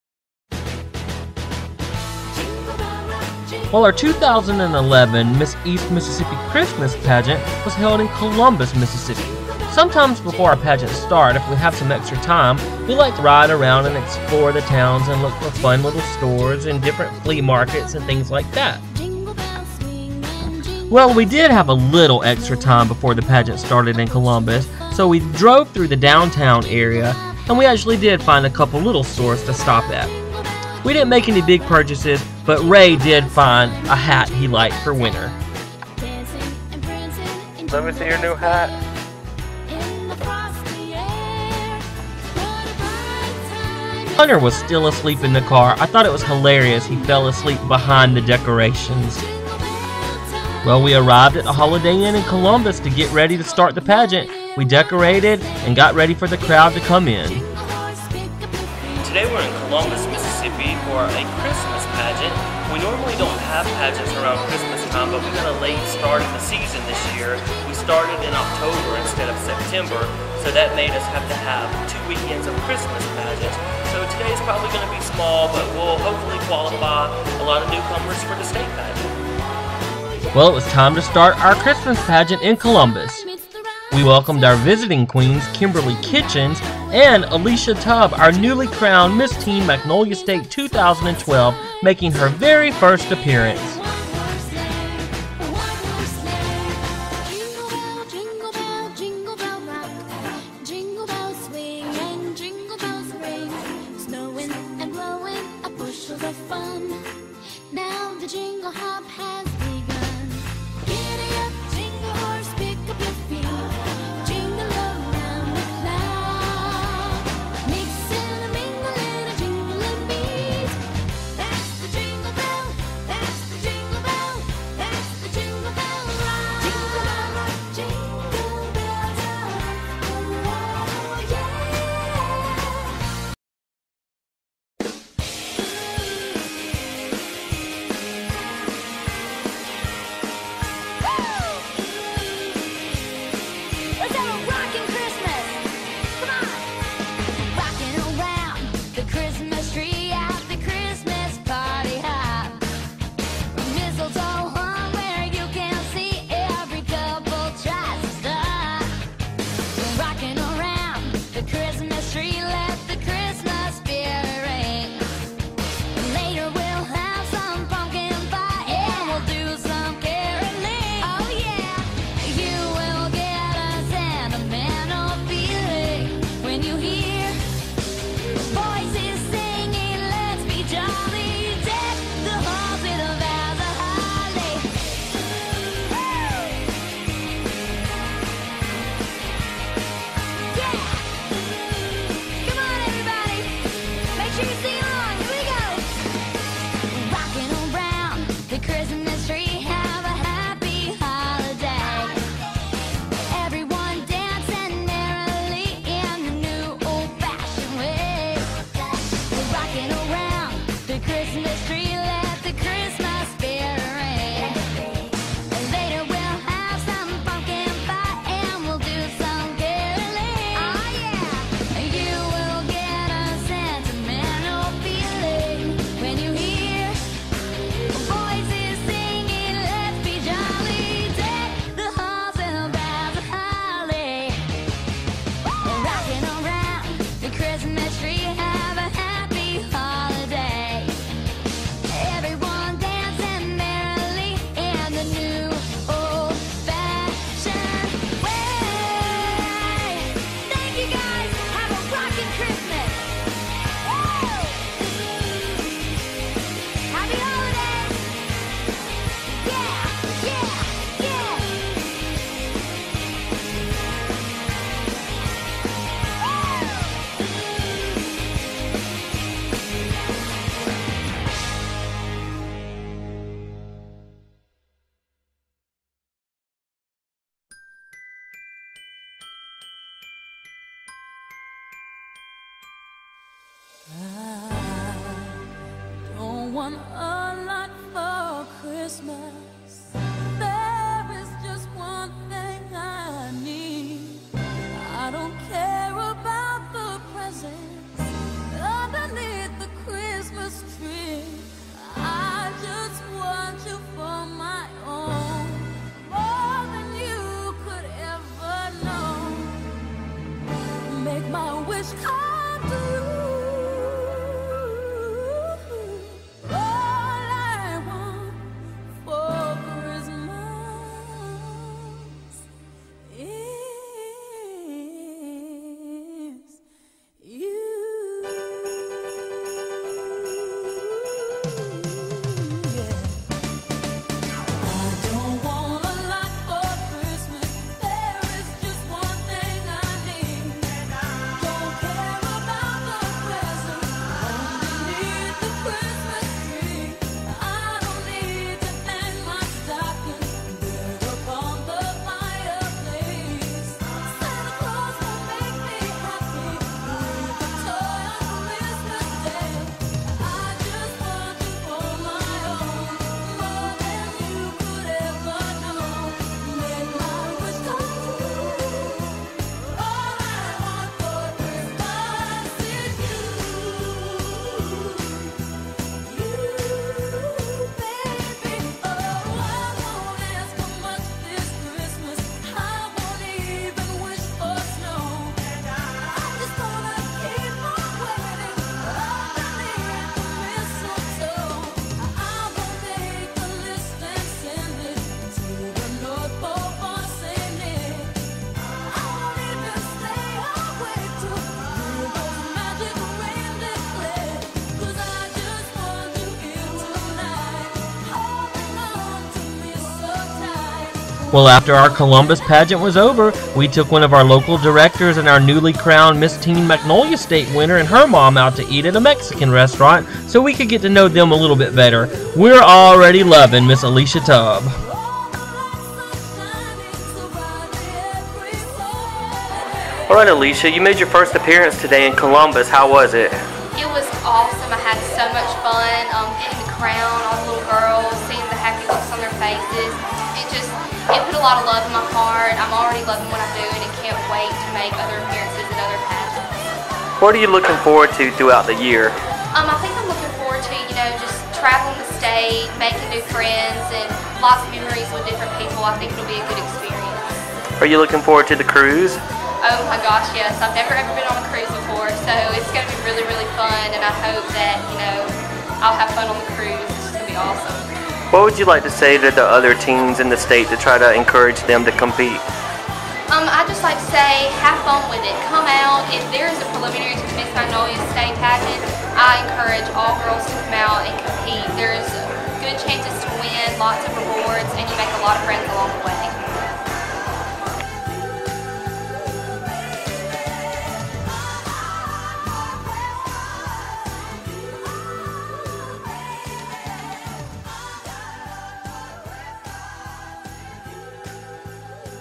Miss Magnolia State East Mississippi Christmas Pageant Dec 10 2011.mpg
Here is a video of the 2011 Miss Magnolia State East Mississippi Christmas Pageant which was held on Dec. 10, 2011 in Columbus, MS. Alicia Tubb, the newly crowned Miss Teen Magnolia State 2012, made her first appearance at this preliminary pageant. For more information on an upcoming Magnolia State Preliminary near your hometown, visit the